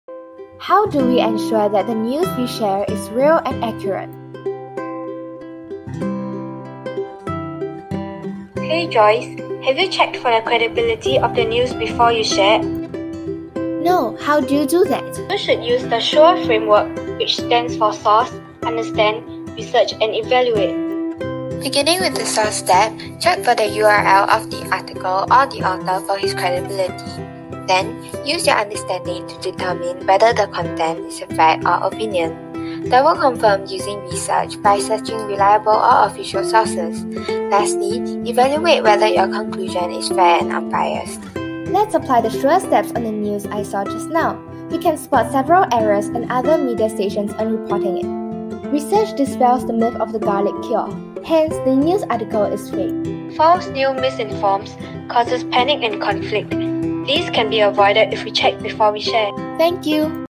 CHIJ St Nicholas Girls’ School– Prove It! Contest 2021 (Top 10)
Organised by the National Library Board, the S.U.R.E. Prove It! Contest 2021 aimed to equip students with information literacy skills in the digital age and apply it to current affairs consumed online.

For more information about the contest and the top 10 school teams, please